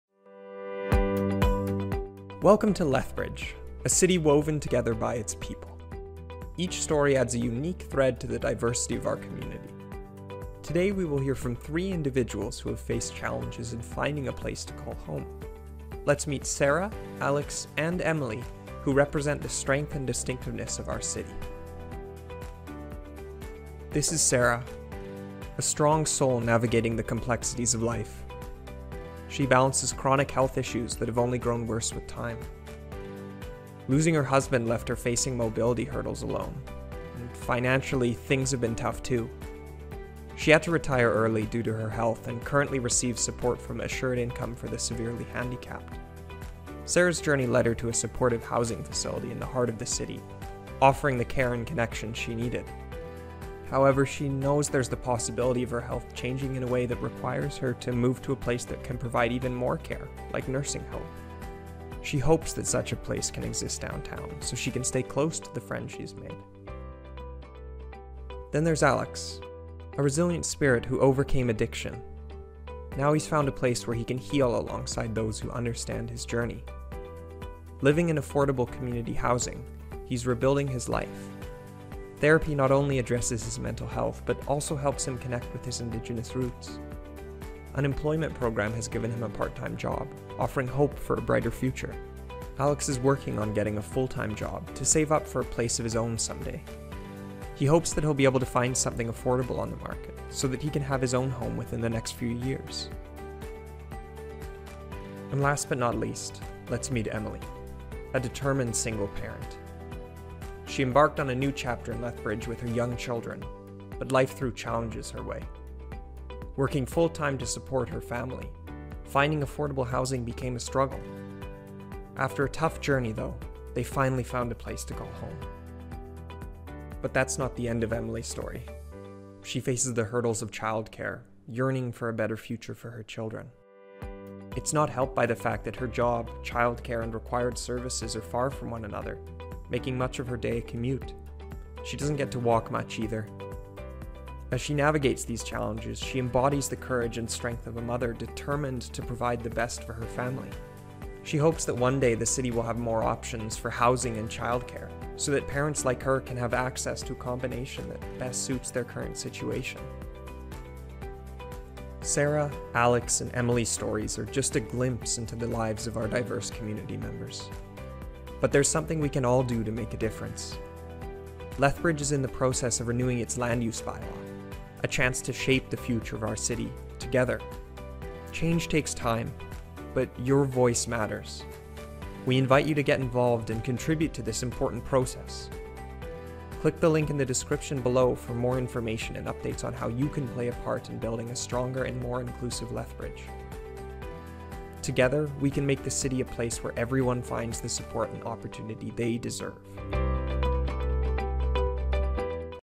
Land Use Bylaw Renewal - Part 4: Get Involved!
The first phase of the Land Use Bylaw Renewal project focuses on housing with social supports and social services. Watch this video on residents who may need these type of services and complete the survey at getinvolvedlethbridge.ca/lub.